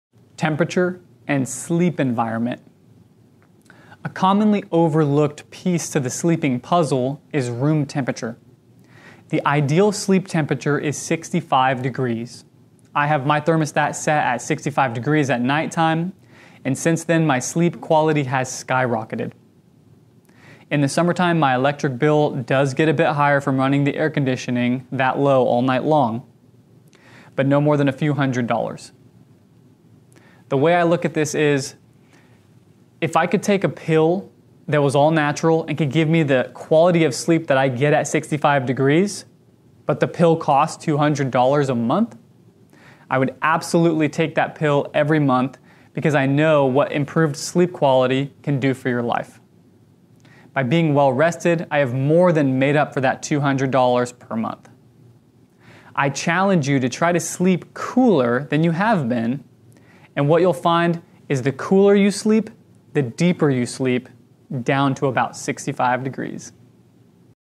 The One Thing More Important Than Mattress Quality - The Basics (31/49)
Good sleep is all about a good mattress right? Wrong. There's many other factors to good sleep and in this video I'll go over the basics to the most important one.